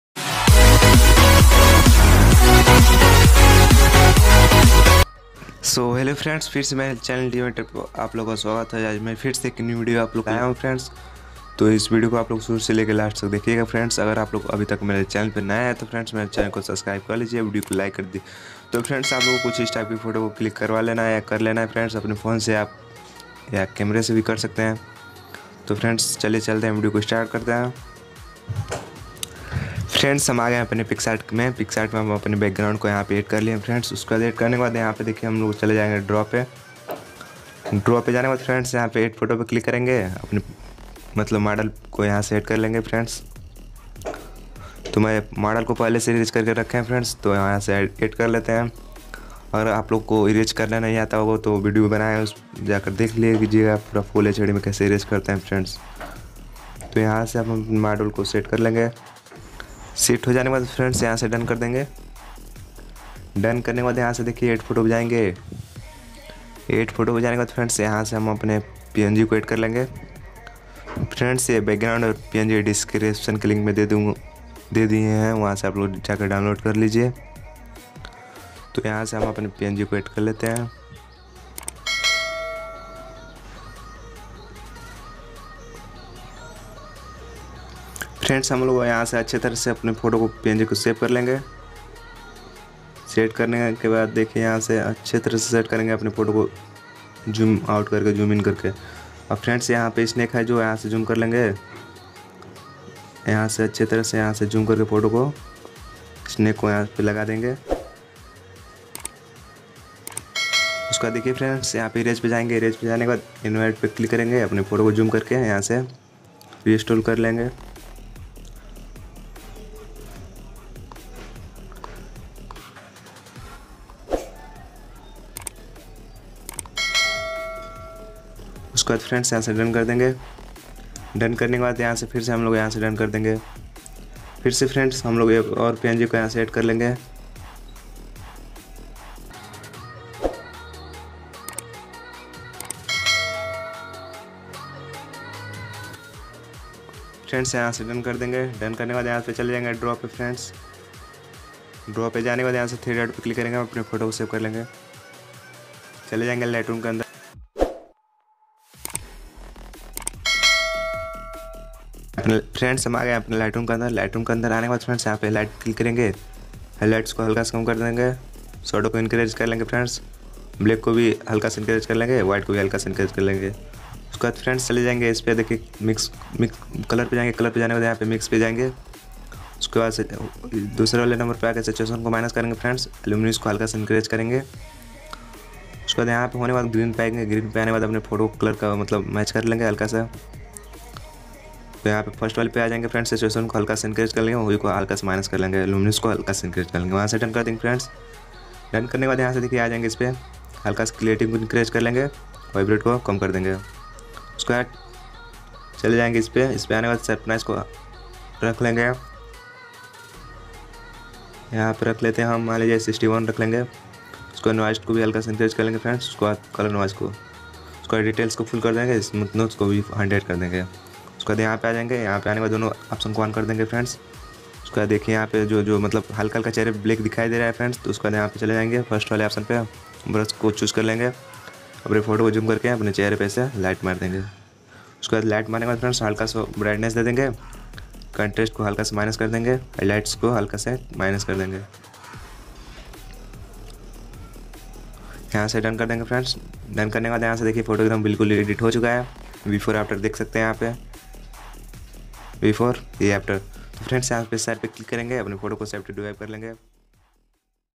Snake🔥 New Concept Photo Editing In Picsart | Picsart Editing Tutorial | Lightroom Editing .
तो दोस्तो मै आज आ लोगो के लिए न्यू वीडियो लाया हु जो
Backround Change Kaise Karte Hai

Backround Change .
तो ये video आप लोगो लास्ट तक देखे और video को share करे।

How to change backround |
picsart se backround change kaise karte hai |
photo editing